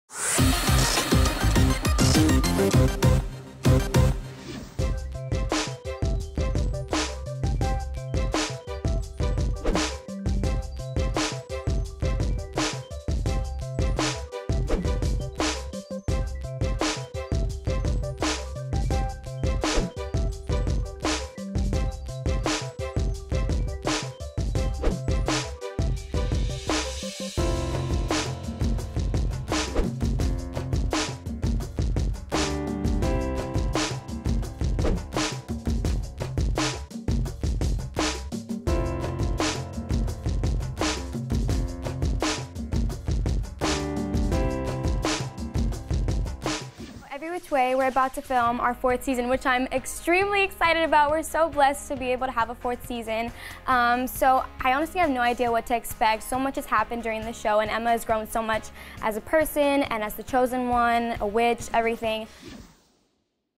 "Every Witch Way" star, Paola Andino Facts + Bonus Surprise!
Let’s be real for a minute, we’re going to miss Paola Andino like crazy on Every Witch Way, now that the show has officially ended. But that doesn’t mean we can’t keep the magic alive!

Down below, we uncovered a few facts about Paola that you might not have known. For example…

Did you know that Paola started taking acting classes when she was just 10 years old?
Or that she danced back in Texas starting at a very young age?
Did you see that one Grey’s Anatomy episode that Paola guest starred on?
...or how about her role in the movie Beyond the Blackboard?
She started all of this after moving from Bayamon, Puerto Rico to Dallas, Texas when she was just three years old!
SO! How many of those did you know? Check out the video down below, and don’t miss the hidden surprise that only true fans should be able to access. And make sure you keep checking back with us as we bring you even more awesome facts about your favorite stars!